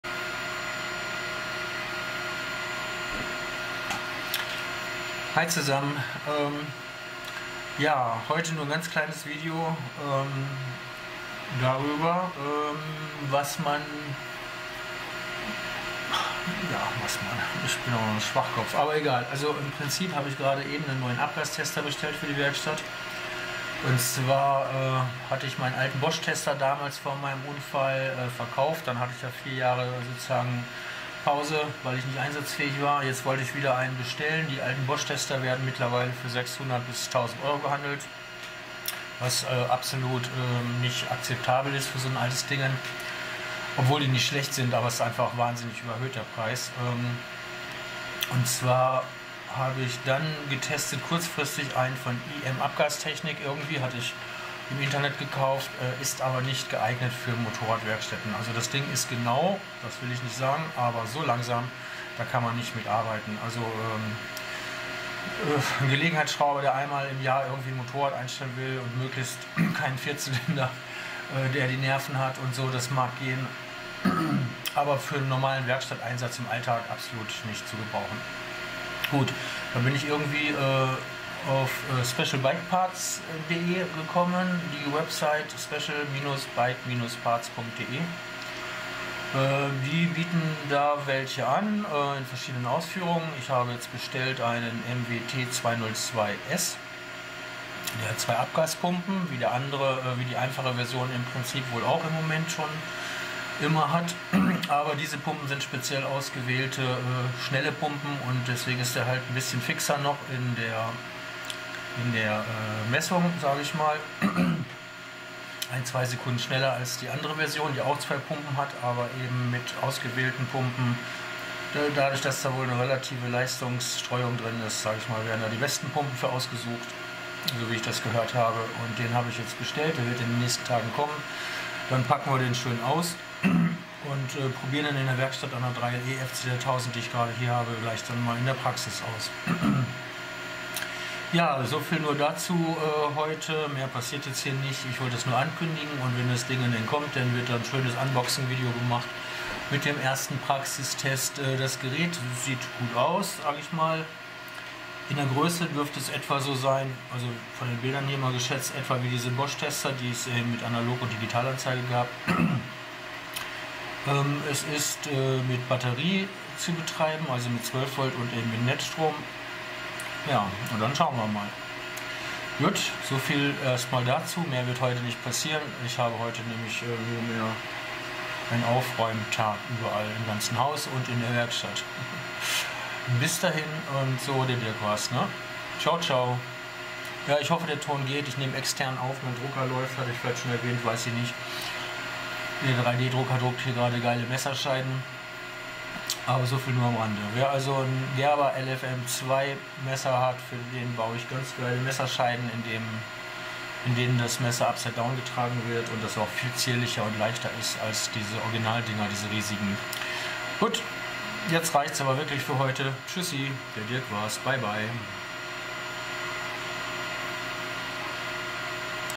Abgastester (CO) von Fa. Special-Bike-Parts.de - Sorry für HG Geräusche - ist mein 3D Drucker :-)
Habe gerade diesen Abgastester für die Werkstatt bestellt.
Wird in den nächsten Tagen geliefert und dann machen wir ein Unboxing und einen ersten Praxistest an einer FZR1000 3LE Yamaha.
Gibt es bei: Fa. Special-Bike-Parts.de.
Die haben viele schöne Sachen :-)